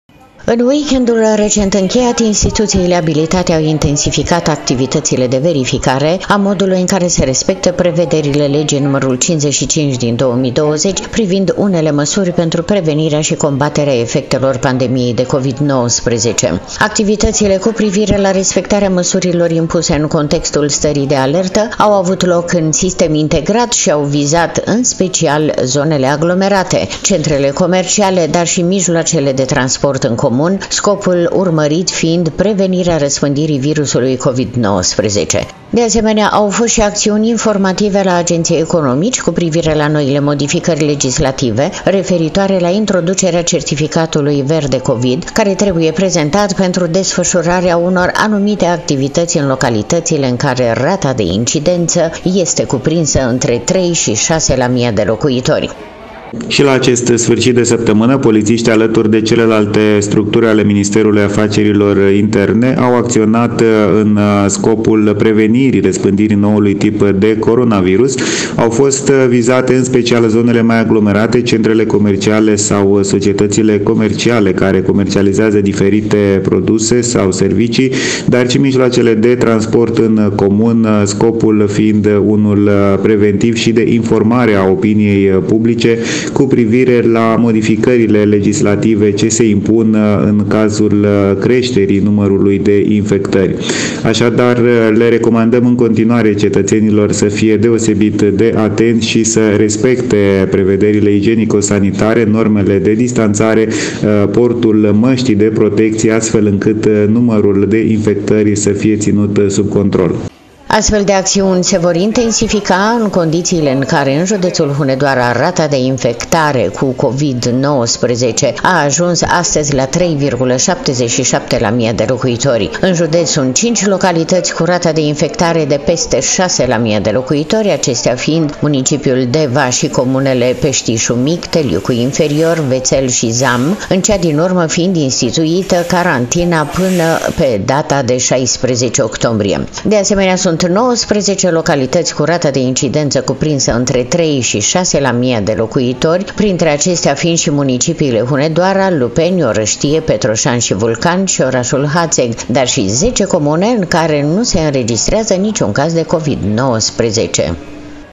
ACȚIUNI ALE POLIȚIEI PENTRU PREVENIREA RĂSPÂNDIRII COVID 19
În week-end-ul recent încheiat, instituțiile abilitate au intensificat activitățile de verificare a modului în care se respectă prevederile Legii nr. 55 din 2020 privind unele măsuri pentru prevenirea și combaterea efectelor pandemiei de COVID-19. Activităţile cu privire la respectarea măsurilor impuse în contextul stării de alertă au avut loc în sistem integrat şi au vizat, în special, zonele aglomerate, centre comerciale, dar şi mijloacele de transport în comun, scopul urmărit fiind prevenirea răspândirii virusului Covid-19. De asemenea, au fost și acțiuni informative la agenții economici, cu privire la noile modificări legislative referitoare la introducerea Certificatului verde  COVID, care trebuie prezentat pentru desfășurarea unor anumite activități în localitățile în care rata de incidență este cuprinsă între trei și șase la mia de locuitori.